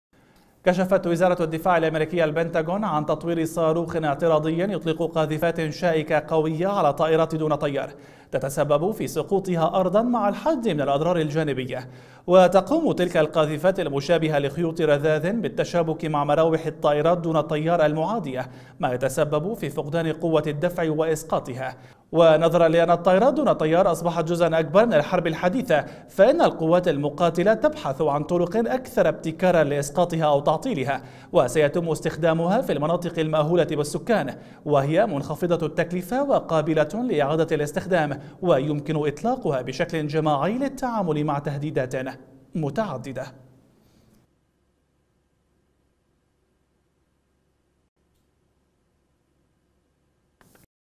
"البنتاغون" يطور تقنية جديدة لإسقاط الطائرات المسيرة في المناطق السكنية
كشفت وزارة الدفاع الأمريكية البنتاغون عن تطوير صاروخ اعتراضي يطلق قاذفات شائكة قوية على الطائرات دون طيار، تتسبب في سقوطها أرضاً مع الحد من الأضرار الجانبية. وتقوم تلك القاذفات المشابهة لخيوط رذاذ بالتشابك مع مراوح الطائرات دون طيار المعادية؛ ما يتسبب في فقدان قوة الدفع وإسقاطها. ونظرا لأن الطائرات دون طيار أصبحت جزءا أكبر من الحرب الحديثة، فإن القوات المقاتلة تبتكر طرقا أكثر ابتكاراً لإسقاطها أو تعطيلها. وسيتم استخدامها في المناطق المأهولة بالسكان، وهي منخفضة التكلفة وقابلة لإعادة الاستخدام، ويمكن إطلاقها بشكل جماعي للتعامل مع تهديدات متعددة.